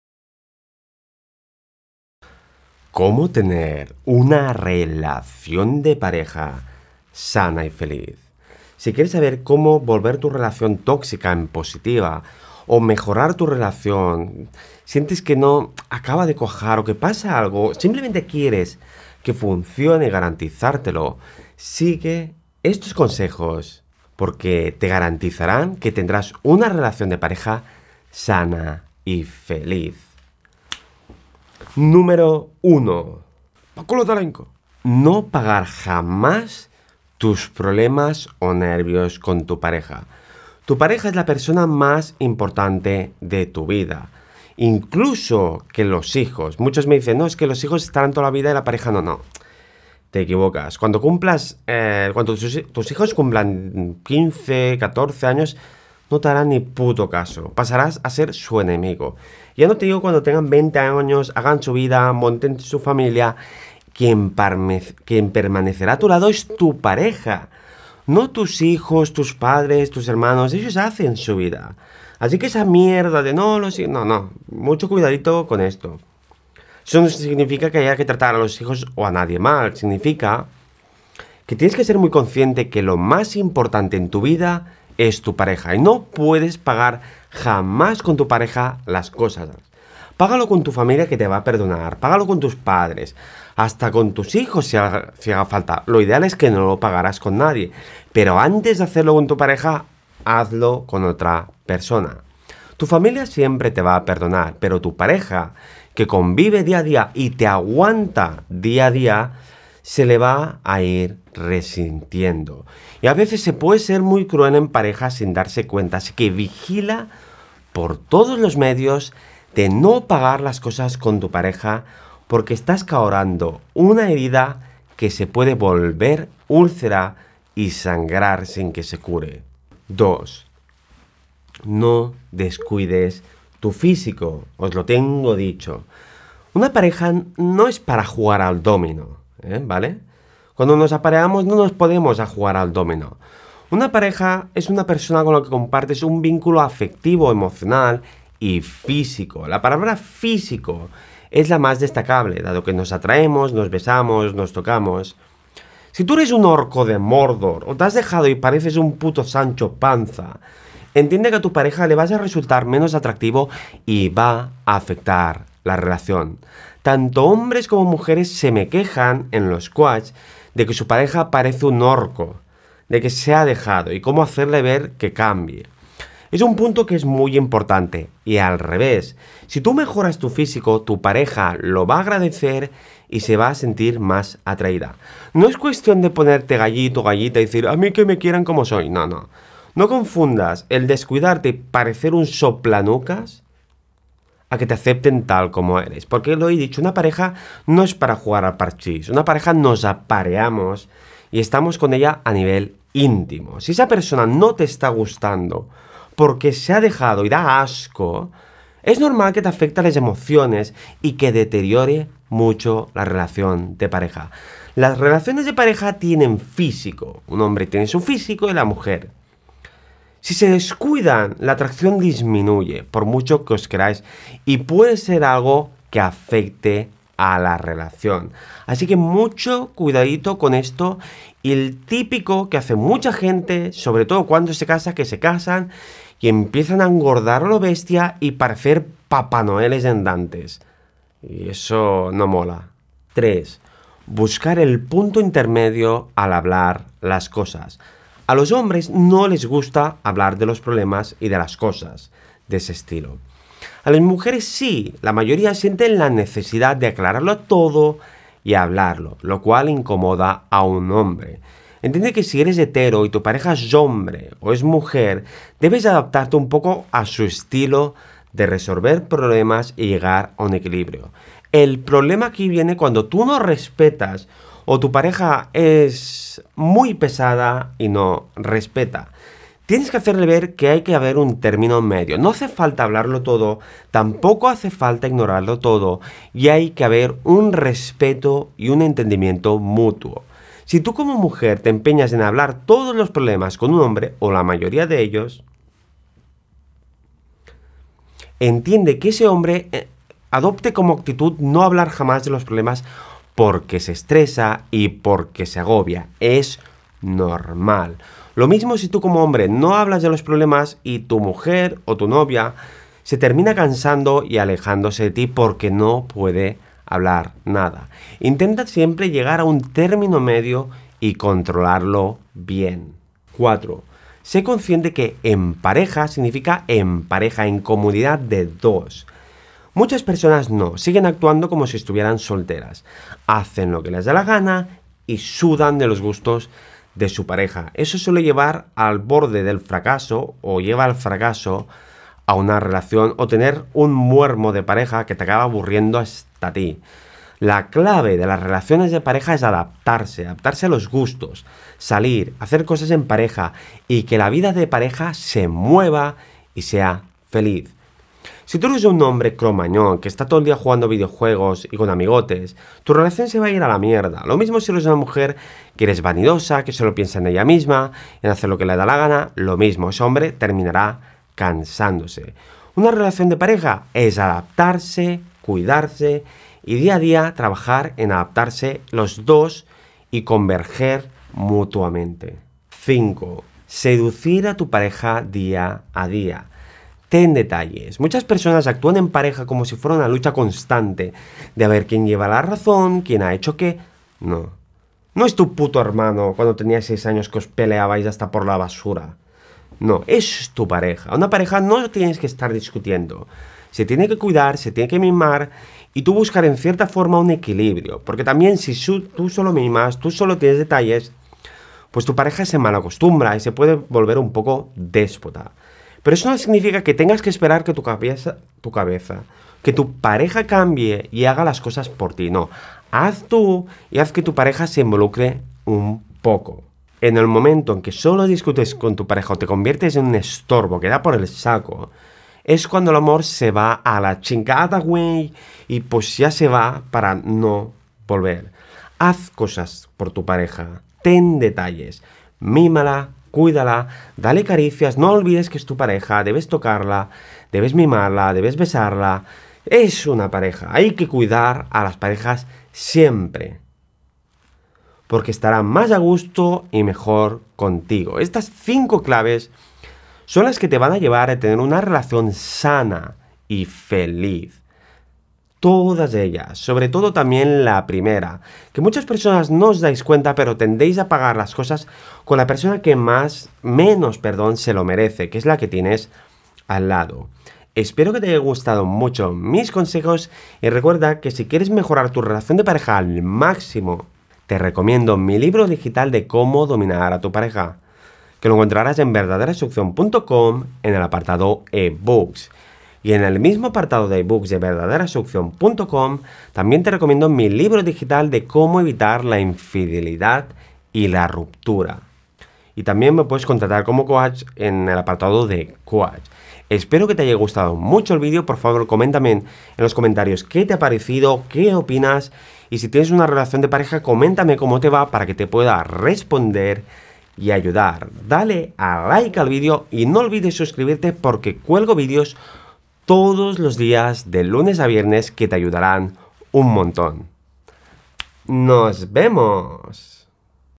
Como Tener Una Relación De Pareja Sana Y Feliz 🎈
Esta es tu oportunidad para decir adios a los problemas de pareja y comenzar a tener una relación de pareja sana y feliz.

La pareja perfecta no existe, pero si que existe la pareja feliz o la pareja sana. No hay que buscar la perfección, simplemente hay que buscar llevar una relación de pareja sana y feliz para que puedas estar a gusto y disfrutar.

Los conflictos de pareja se pueden solucionar si sabes solventarlos. Y la pareja estable puede durar toda la vida si sabes como enfocarlo todo.

Esto es básicamente lo que te enseñamos en el video. Te enseñamos a cuidar el amor de pareja y a tener un matrimonio sano, positivo y feliz.

PRODUCTOS MÍOS QUE TE RECOMIENDO